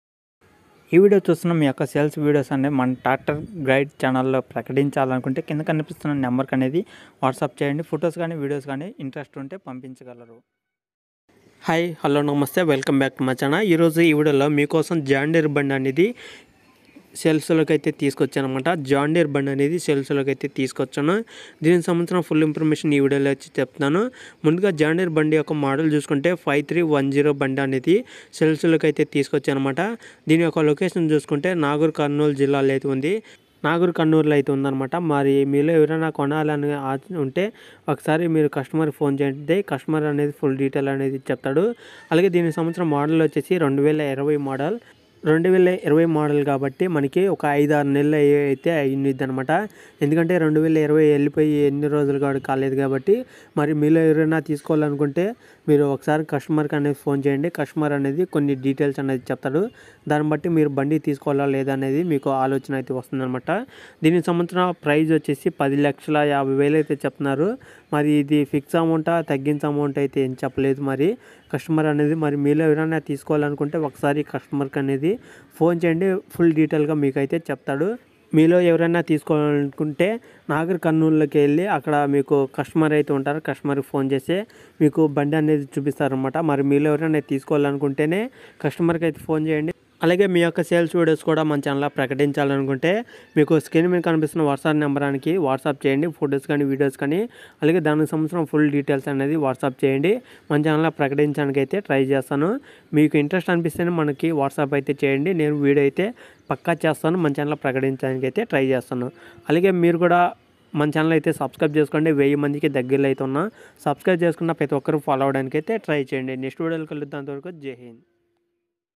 john deere 5310 tractor అమ్మబడును model 2020 || 9502021710 || Tractor Guide
2020 model

Nagar Kurnool district peddakothapally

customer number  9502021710

ఈ వీడియో చూస్తున్న మీ యొక్క సేల్స్ వీడియోస్ కూడా
 మన " Tractor Guide " ఛానల్ లో ప్రకటించాలంటే

ఫొటోస్ మరియు వీడియోస్ పంపించగలరు 😃😃😃